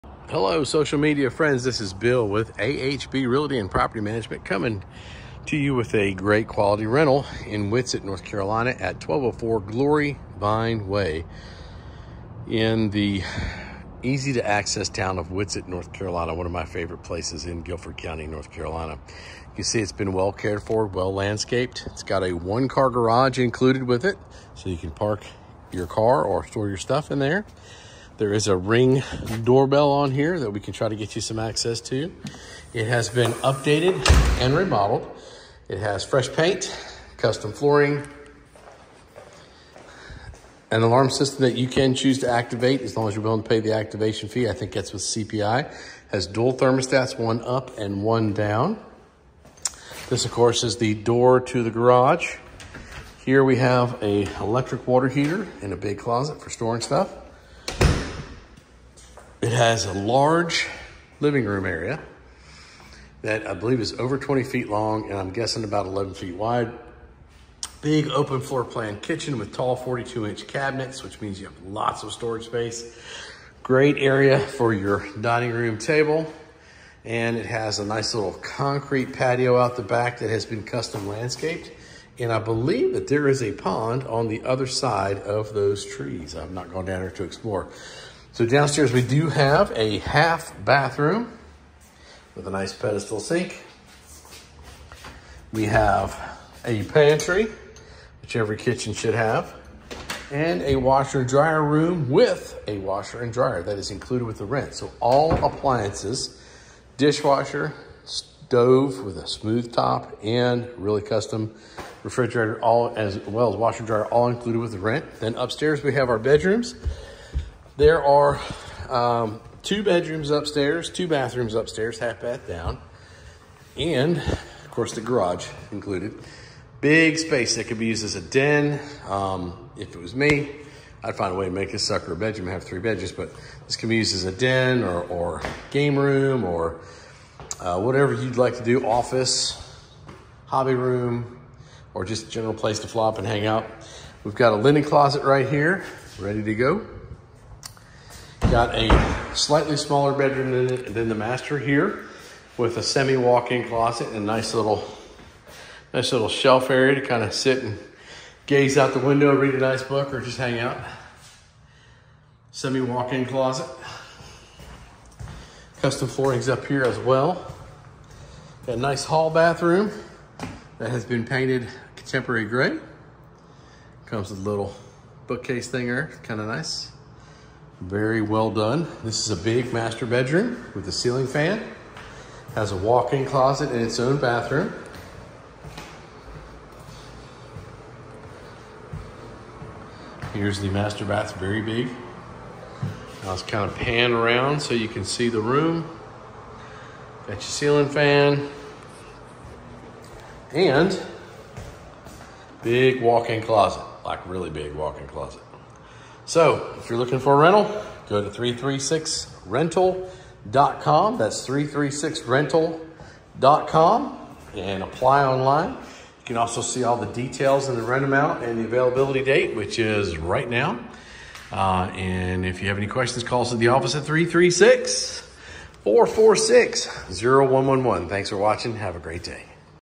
1204 Glory Vine Way, Whitsett, NC 27377 two, bedroom 2 1/2 bath rental home with garage washer&dryer
This is an amazing rental home two bedroom 2 1/2 bath living room, and then includes garage included with the rent is water, sewer, trash, and all landscaping as well landscaped front and rear entryway open floor plan custom floors in amazing shape, central heat and air duel, zone electric water heater. And a one car garage this is a great place to live community pool also. ￼